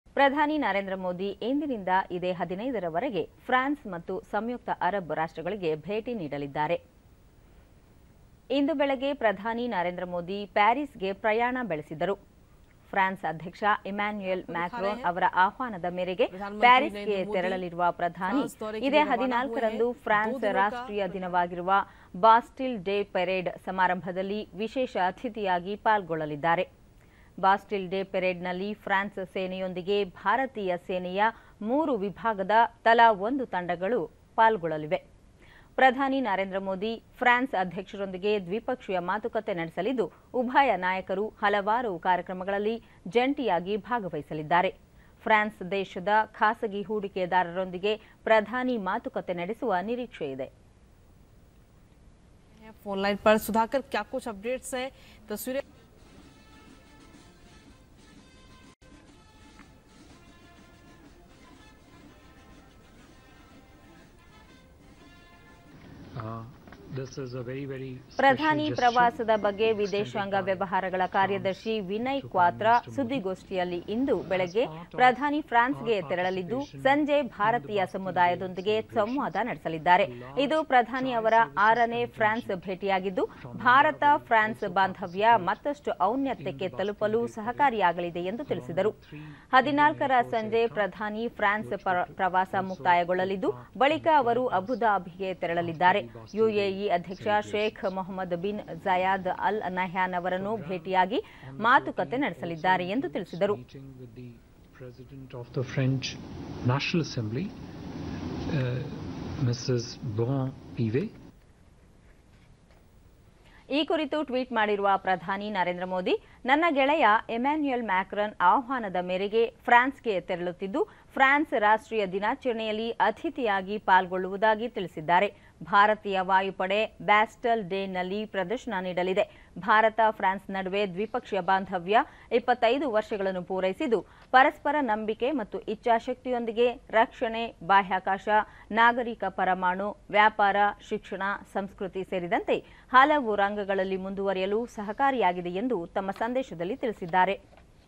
ಪ್ರಧಾನಿ ನರೇಂದ್ರ ಮೋದಿ ಇಂದಿನಿಂದ 15ರವರೆಗೆ ಫ್ರಾನ್ಸ್,ಯುಎಇ ಭೇಟಿ ..
ಪ್ರಧಾನಿ ನರೇಂದ್ರ ಮೋದಿ, ಇಂದಿನಿಂದ ಇದೇ 15ರವರೆಗೆ  ಫ್ರಾನ್ಸ್ ಮತ್ತು ಸಂಯುಕ್ತ  ಅರಬ್  ರಾಷ್ಟ್ರಗಳಿಗೆ   ಭೇಟಿ ನೀಡಲಿದ್ದಾರೆ.
ಇಂದು  ಬೆಳಗ್ಗೆ  ಪ್ರಧಾನಿ ನರೇಂದ್ರ ಮೋದಿ  ಪ್ಯಾರಿಸ್ ಗೆ  ಪ್ರಯಾಣ ಬೆಳೆಸಿದರು.
ಫ್ರಾನ್ಸ್ ಅಧ್ಯಕ್ಷ  ಇಮ್ಯಾನ್ಯುಯೆಲ್ ಮ್ಯಾಕ್ರೊನ್  ಅವರ ಆಹ್ವಾನದ ಮೇರೆಗೆ  ಪ್ಯಾರಿಸ್ ಗೆ  ತೆರಳಲಿರುವ  ಪ್ರಧಾನಿ, ಇದೇ 14ರಂದು  ಫ್ರಾನ್ಸ್ ರಾಷ್ಟ್ರೀಯ ದಿನವಾಗಿರುವ  ಬಾಸ್ಟಿಲ್ ಡೇ  ಪರೇಡ್  ಸಮಾರಂಭದಲ್ಲಿ  ವಿಶೇಷ ಅತಿಥಿಯಾಗಿ  ಪಾಲ್ಗೊಳ್ಳಲಿದ್ದಾರೆ.
ಬಾಸ್ಟಿಲ್  ಡೇ  ಪರೇಡ್  ನಲ್ಲಿ  ಫ್ರಾನ್ಸ್ ಸೇನೆಯೊಂದಿಗೆ  ಭಾರತೀಯ ಸೇನೆಯ  ಮೂರೂ ವಿಭಾಗದ  ತಲಾ ಒಂದು  ತಂಡಗಳು  ಪಾಲ್ಗೊಳ್ಳಲಿವೆ.
ಪ್ರಧಾನಿ ನರೇಂದ್ರ ಮೋದಿ, ಫ್ರಾನ್ಸ್ ಅಧ್ಯಕ್ಷರೊಂದಿಗೆ  ದ್ವಿಪಕ್ಷೀಯ ಮಾತುಕತೆ ನಡೆಸಲಿದ್ದು, ಉಭಯ ನಾಯಕರು  ಹಲವಾರು  ಕಾರ್ಯಕ್ರಮಗಳಲ್ಲಿ  ಜಂಟಿಯಾಗಿ ಭಾಗವಹಿಸಲಿದ್ದಾರೆ. ಫ್ರಾನ್ಸ್ ದೇಶದ  ಖಾಸಗಿ ಹೂಡಿಕೆದಾರರೊಂದಿಗೆ  ಪ್ರಧಾನಿ ಮಾತುಕತೆ ನಡೆಸುವ ನಿರೀಕ್ಷೆಯಿದೆ.
(WATCH)
ಪ್ರಧಾನಿ ಪ್ರವಾಸದ ಬಗ್ಗೆ  ವಿದೇಶಾಂಗ ವ್ಯವಹಾರಗಳ ಕಾರ್ಯದರ್ಶಿ  ವಿನಯ್ ಕ್ವಾತ್ರ  ಸುದ್ದಿಗೋಷ್ಟಿಯಲ್ಲಿ, ಇಂದು ಬೆಳಗ್ಗೆ  ಪ್ರಧಾನಿ ಫ್ರಾನ್ಸ್ ಗೆ  ತೆರಳಲಿದ್ದು,  ಸಂಜೆ  ಭಾರತೀಯ ಸಮುದಾಯದೊಂದಿಗೆ  ಸಂವಾದ ನಡೆಸಲಿದ್ದಾರೆ. ಇದು  ಪ್ರಧಾನಿ ಅವರ  6ನೇ ಫ್ರಾನ್ಸ್  ಭೇಟಿಯಾಗಿದ್ದು, ಭಾರತ- ಫ್ರಾನ್ಸ್ ಬಾಂಧವ್ಯ  ಮತ್ತಷ್ಟು  ಔನ್ನತ್ಯಕ್ಕೆ  ತಲುಪಲು ಸಹಕಾರಿಯಾಗಲಿದೆ ಎಂದು ತಿಳಿಸಿದರು.
14ರ  ಸಂಜೆ  ಪ್ರಧಾನಿ   ಫ್ರಾನ್ಸ್ ಪ್ರವಾಸ ಮುಕ್ತಾಯಗೊಳ್ಳಲಿದ್ದು, ಬಳಿಕ ಅವರು  ಅಬುಧಾಬಿಗೆ  ತೆರಳಲಿದ್ದಾರೆ. U A E ಅಧ್ಯಕ್ಷ  ಶೇಖ್ ಮಹಮ್ಮದ್ ಬಿನ್  ಝಾಯೇದ್  ಅಲ್ ನಹ್ಯಾನ್  ಅವರನ್ನು  ಭೇಟಿಯಾಗಿ  ಮಾತುಕತೆ  ನಡೆಸಲಿದ್ದಾರೆ ಎಂದು ತಿಳಿಸಿದರು. (3.10)  (HOLD)
ಈ ಕುರಿತು  ಟ್ವೀಟ್ ಮಾಡಿರುವ  ಪ್ರಧಾನಿ  ನರೇಂದ್ರ ಮೋದಿ, ನನ್ನ ಗೆಳೆಯ  ಎಮ್ಯಾನುಯಲ್ ಮ್ಯಾಕ್ರಾನ್  ಆಹ್ವಾನದ ಮೇರೆಗೆ  ಫ್ರಾನ್ಸ್ ಗೆ ತೆರಳುತ್ತಿದ್ದು, ಫ್ರಾನ್ಸ್  ರಾಷ್ಟ್ರೀಯ ದಿನಾಚರಣೆಯಲ್ಲಿ  ಅತಿಥಿಯಾಗಿ  ಪಾಲ್ಗೊಳ್ಳುವುದಾಗಿ  ತಿಳಿಸಿದ್ದಾರೆ.
ಭಾರತೀಯ  ವಾಯುಪಡೆ  ಬ್ಯಾಸ್ಟಲ್  ಡೇ ನಲ್ಲಿ  ಪ್ರದರ್ಶನ ನೀಡಲಿದೆ. ಭಾರತ-ಫ್ರಾನ್ಸ್ ನಡುವೆ  ದ್ವಿಪಕ್ಷೀಯ ಬಾಂಧವ್ಯ, 25 ವರ್ಷಗಳನ್ನು  ಪೂರೈಸಿದ್ದು, ಪರಸ್ಪರ ನಂಬಿಕೆ ಮತ್ತು ಇಚ್ಛಾಶಕ್ತಿಯೊಂದಿಗೆ  ರಕ್ಷಣೆ, ಬಾಹ್ಯಾಕಾಶ, ನಾಗರಿಕ  ಪರಮಾಣು, ವ್ಯಾಪಾರ, ಶಿಕ್ಷಣ, ಸಂಸ್ಕೃತಿ ಸೇರಿದಂತೆ  ಹಲವು ರಂಗಗಳಲ್ಲಿ  ಮುಂದುವರೆಯಲು  ಸಹಕಾರಿಯಾಗಿದೆ ಎಂದು ತಮ್ಮ ಸಂದೇಶದಲ್ಲಿ ತಿಳಿಸಿದ್ದಾರೆ.